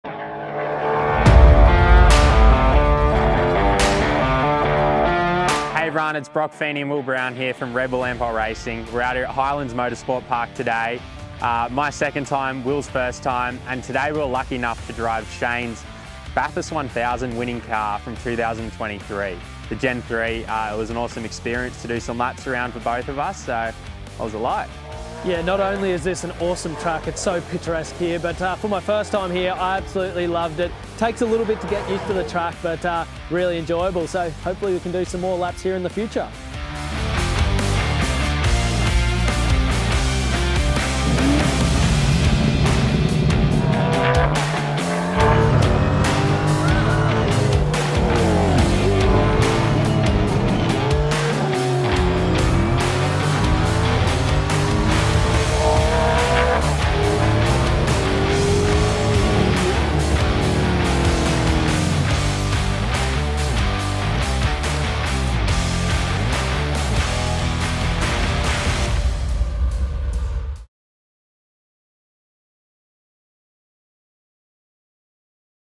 Gen 3 Red Bull Ampol Racing at Highlands, Cromwell, NZ
Shane van Gisbergen & Richie Stanaway Gen 3 Bathurst winning Camaro on track at Highlands driven by Red Bull Ampol Racing Drivers - Will Brown & Broc Feeney.